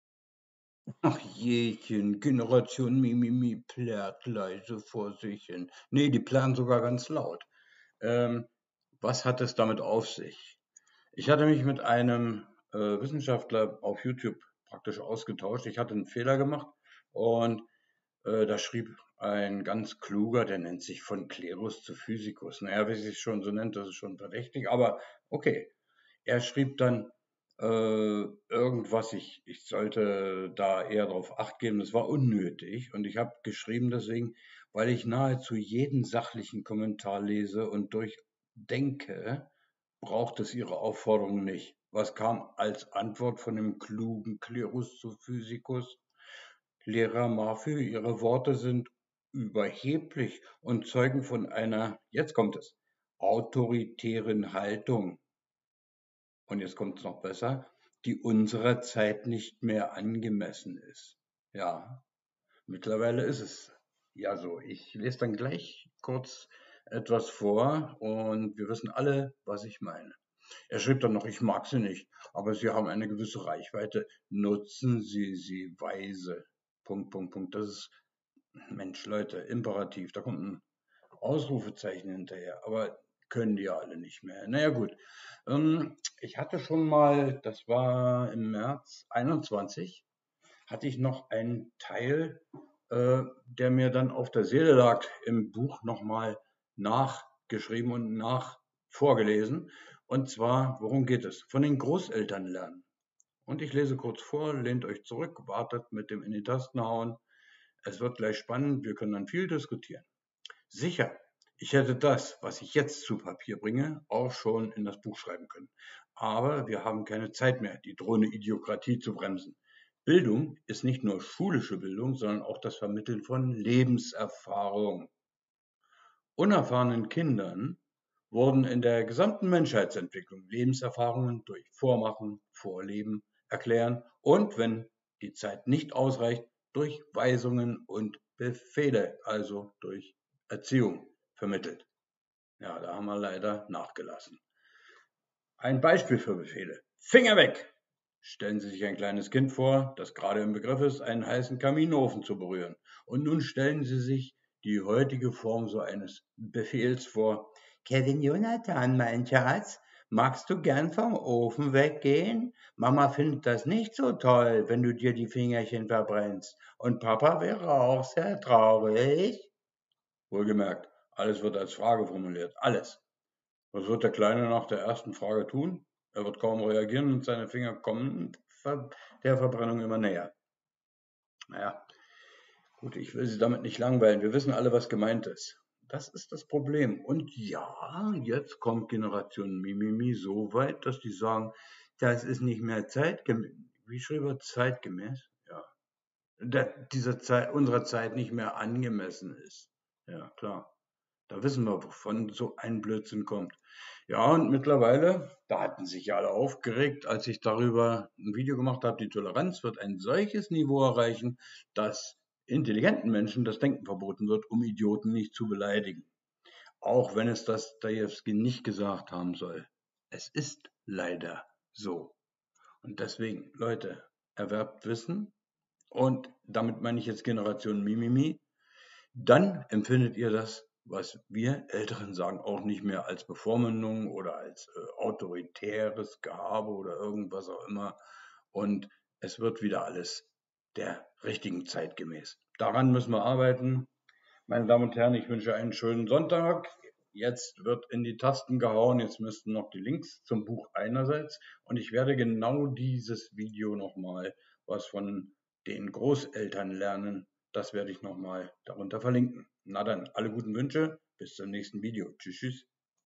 Generation Mimimi, offenes Geplärre - Nachtrag 113 zum Buch “IQ ist nichts, ”
+++ Meinungsfreiheit im MaPhy-Forum +++ Was für traurige Gestalten herumheulen ... Nur zuhören; ich lese vor! Und um YT-Werbung abzuwenden, MEINE Werbung, also der Link zu meinem zweiten Buch "IQ ..."

Björn Köhler